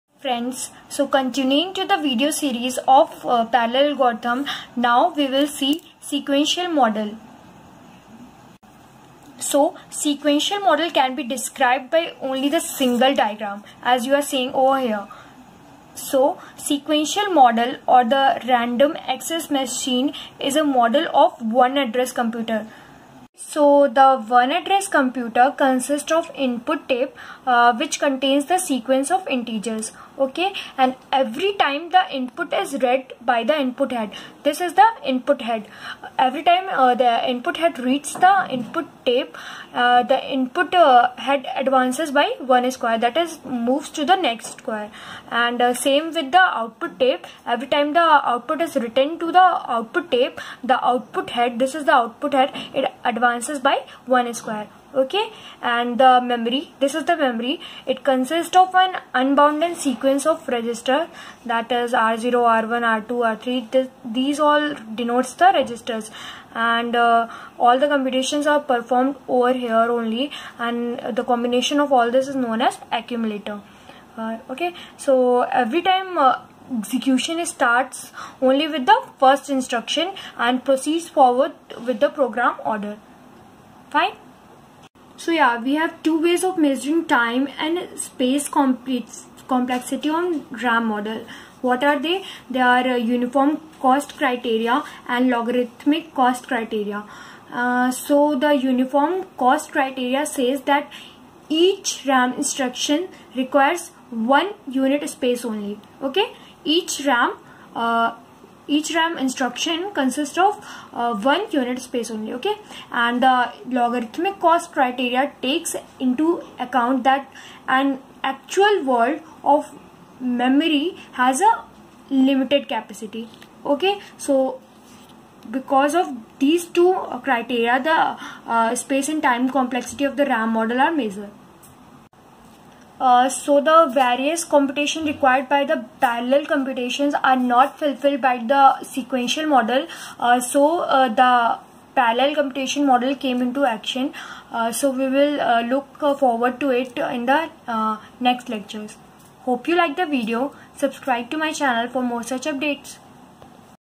Sequential Model in Parallel Algorithm
This video describes the SequentialModel in the ParallelAlgorithm and the need for parallel computational models. Comment down your views about the video or any more suggestions.